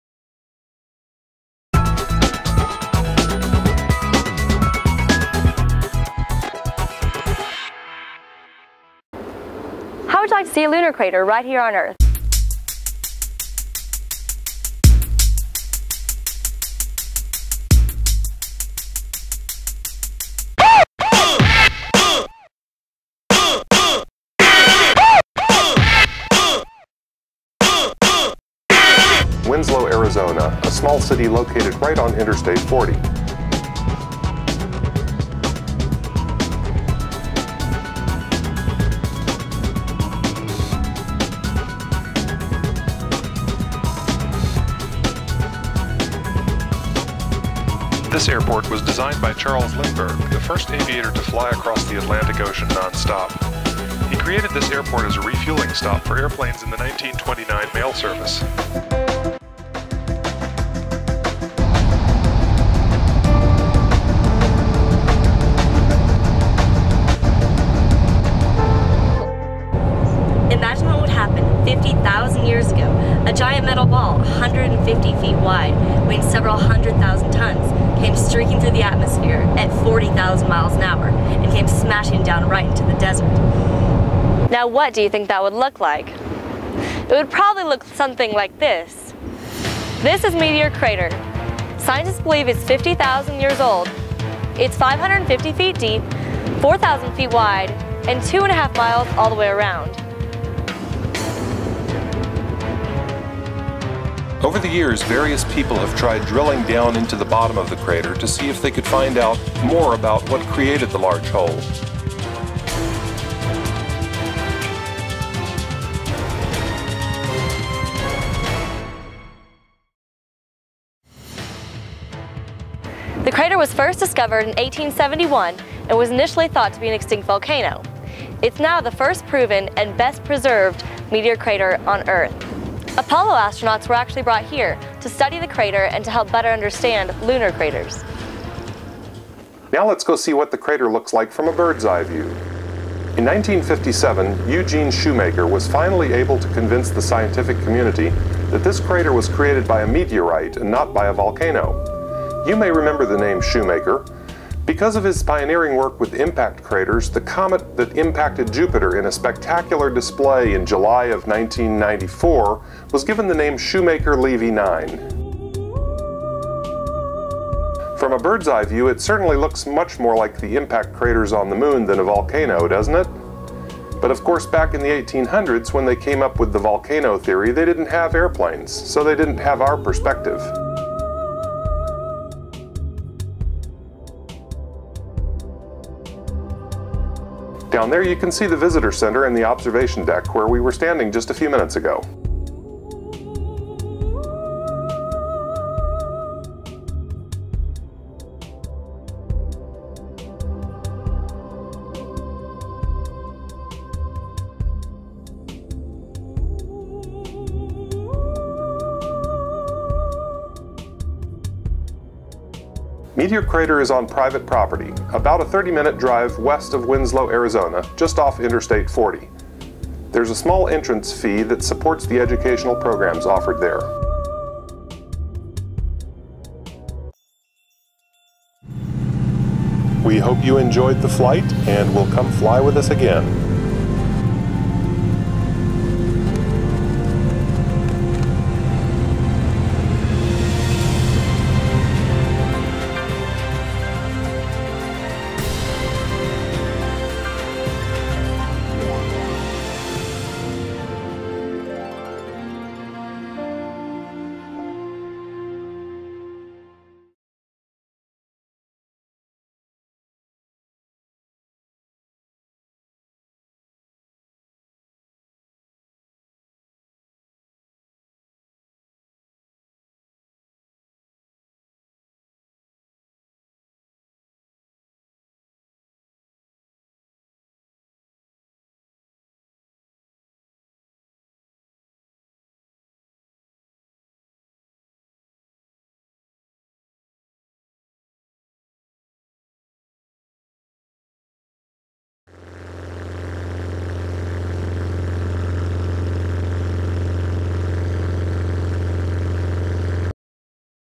Meteor Crater, Winslow Arizona
Come with us as we visit Meteor Crater just outside Winslow Arizona from the ground and from the air